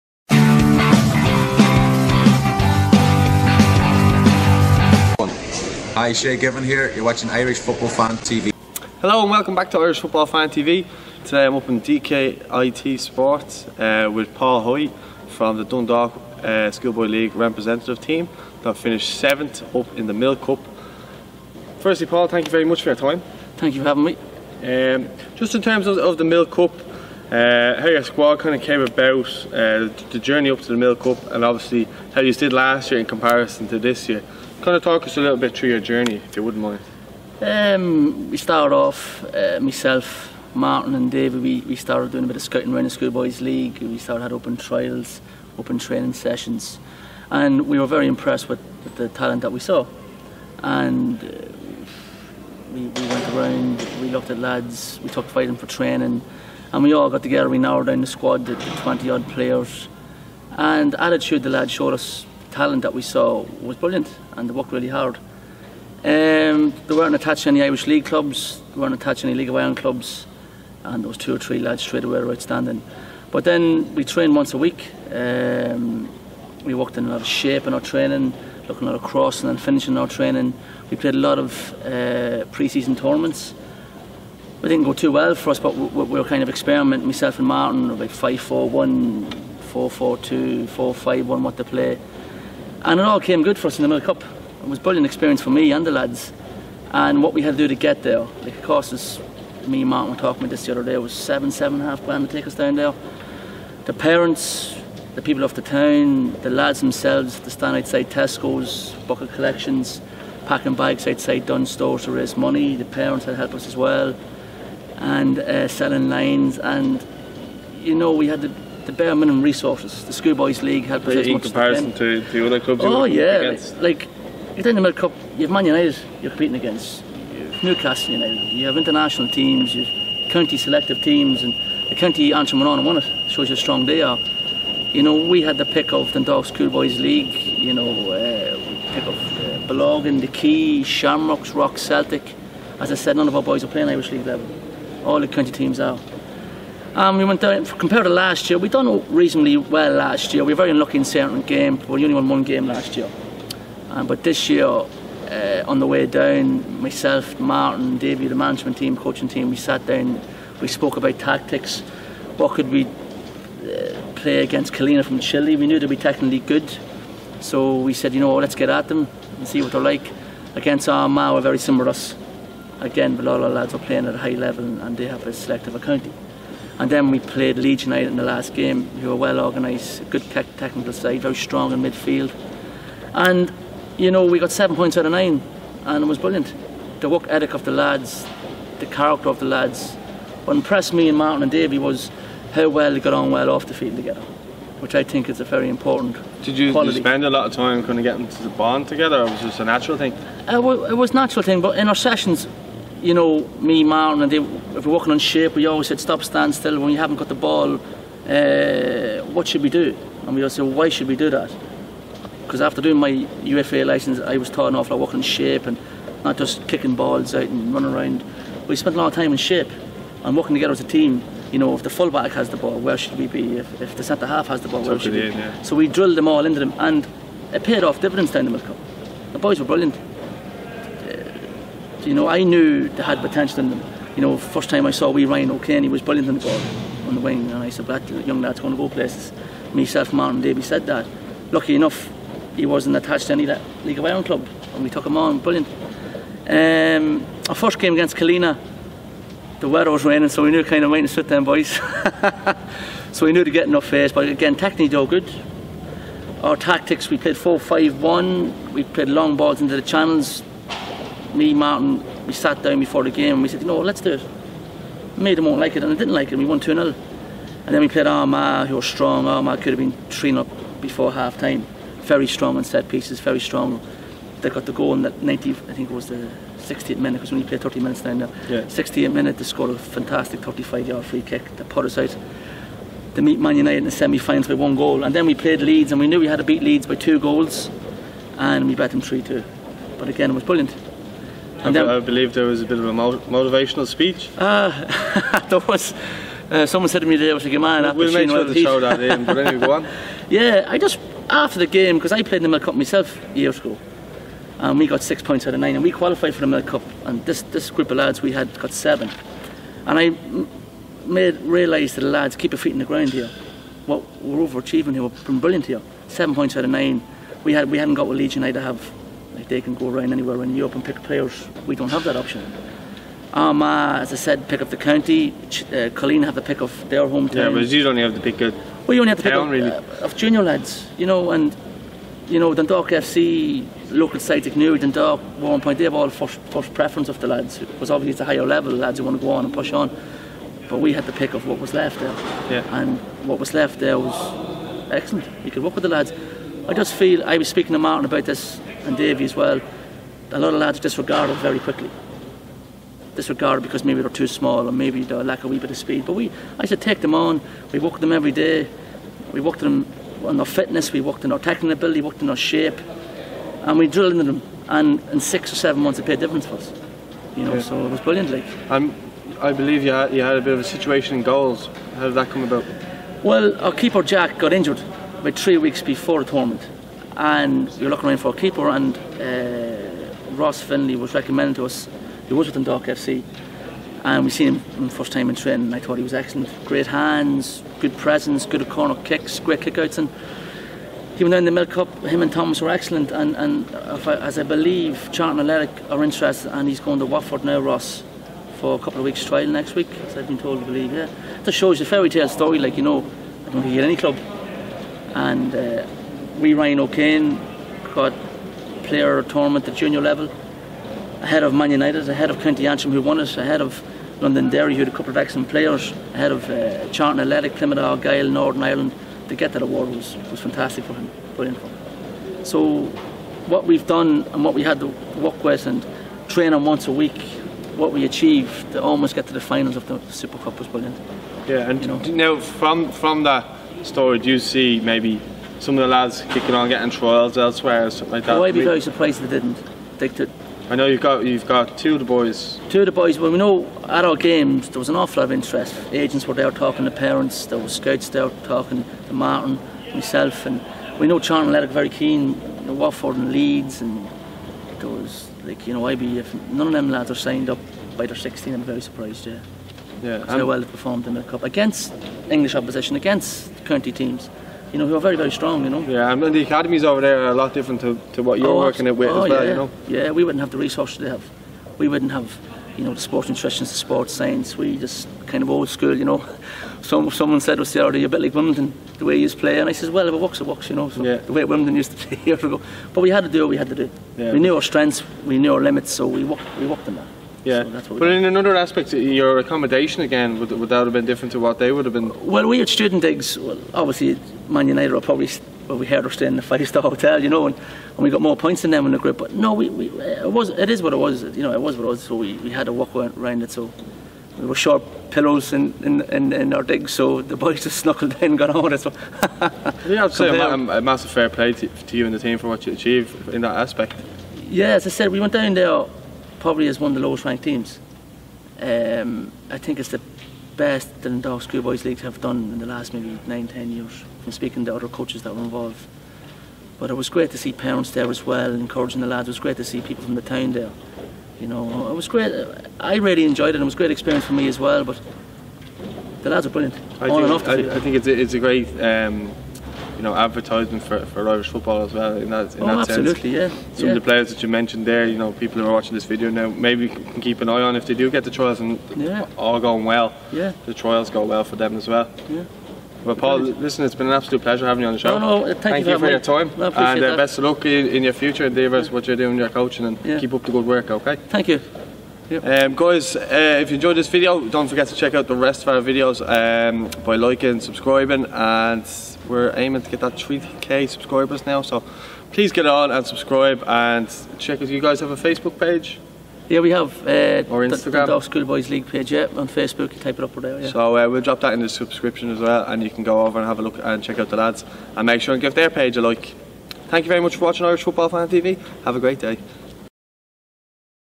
Paul Hoey on Dundalk Schoolboys League on the Milk Cup Run - Incredible Underdog Story
Presenter: Paul Nealon
Guests: Paul Hoey
Video Editing: Paul Nealon
Graphics By: @howey (Aaron Howey)